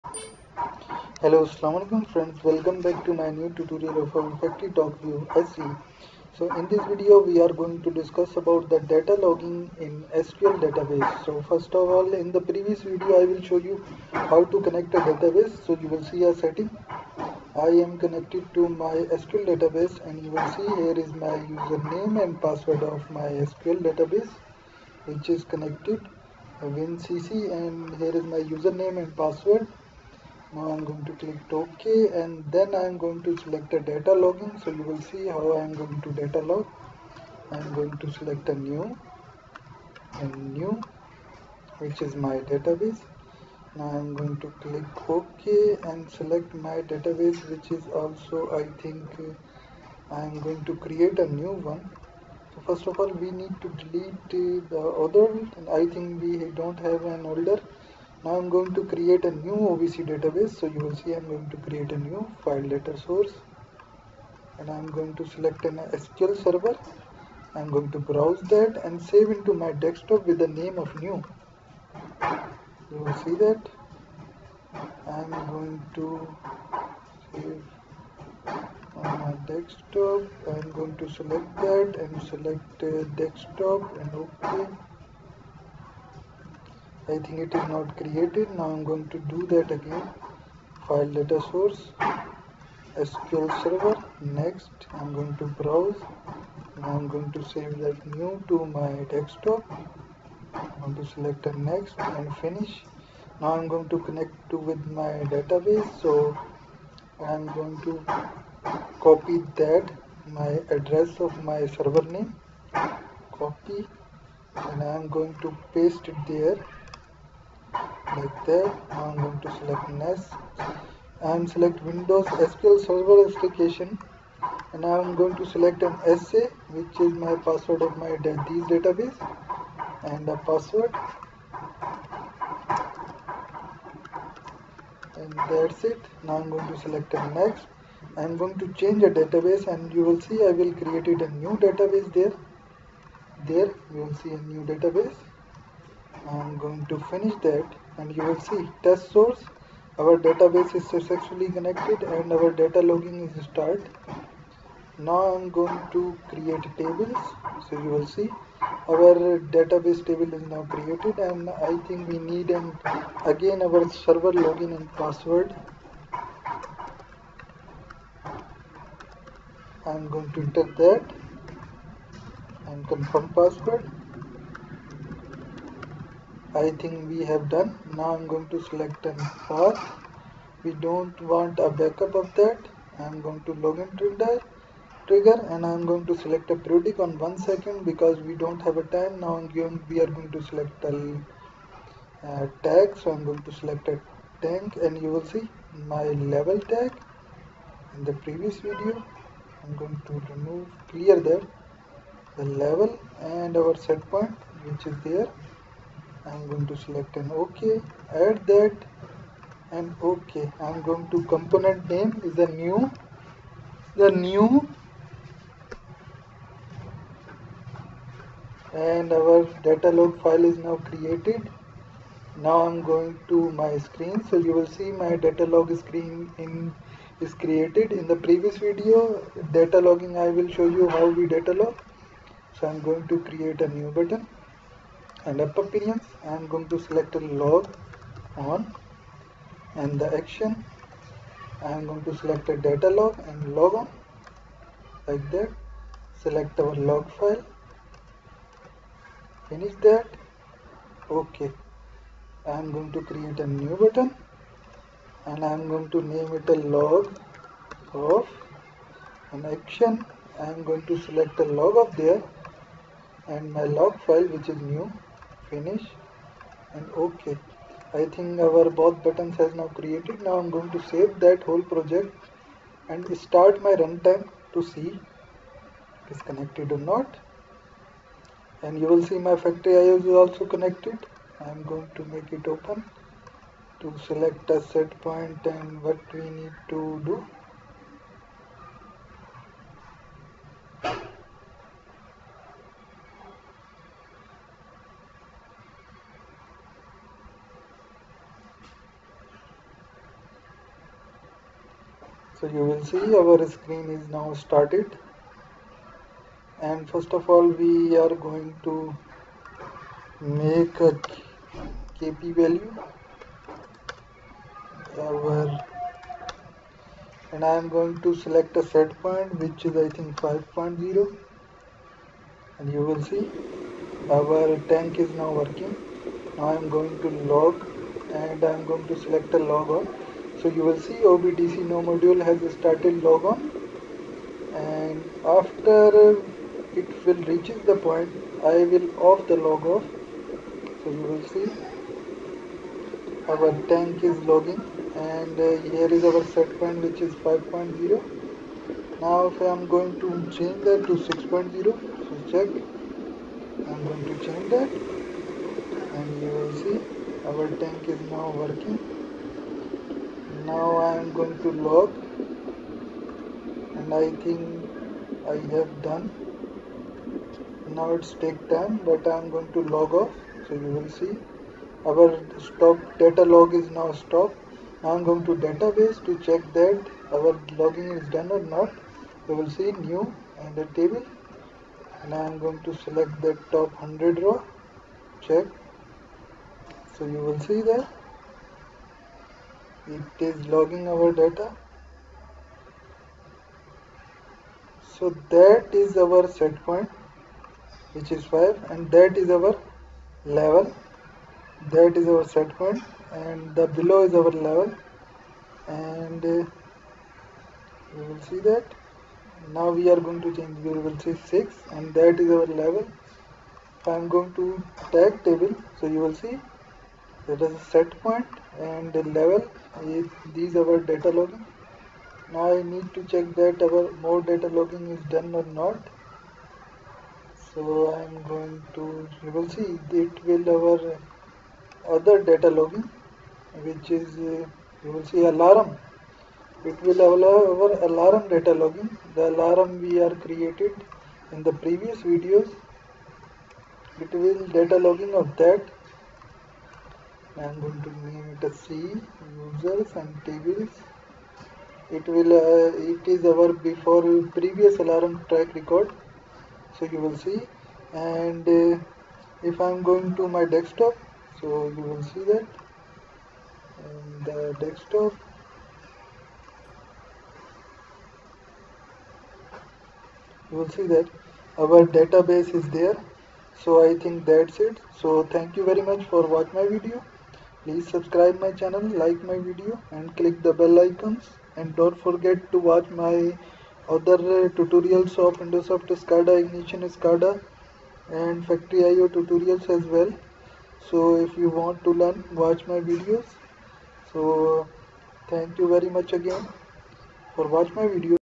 FactoryTalk View | SE | Allen Bradley | SCADA | Tutorial 13 | Data Logging in SQL Database
In this Video You Will Learn About Creating Data Acquisition & Logging of Variable in SQL Database.
If You want any Query Type (The Automation Craze) & Whatsapp me.
If You Want To Learn More & Miss anything Just Watch my Previous Lectures.